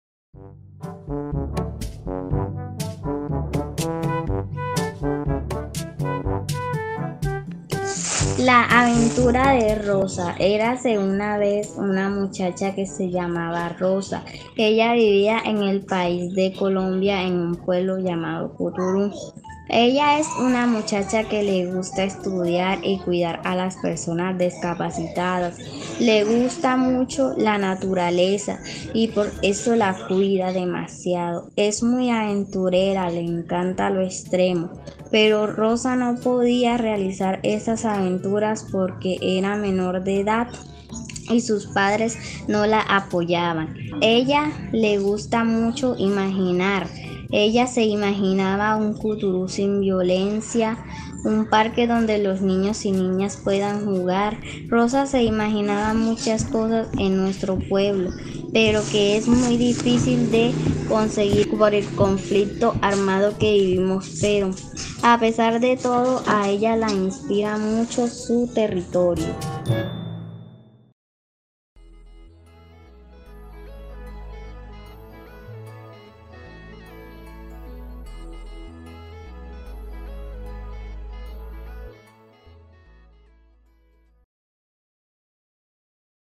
MujeresDeOro - Cuento ganador: "La aventura de Rosa"
Elizabeth Patricia Oviedo Prens, es una de las ganadoras del Concurso de Cuento Corto: Bajo Cauca es todo un cuento.

¡Escucha un fragmento de "La aventura de Rosa" leído por su autora!